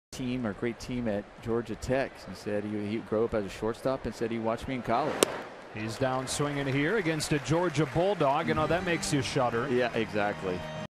ATL@LAD: Wood strikes out Swanson swinging
Alex Wood strikes out Dansby Swanson swinging for the first out of the top of the second inning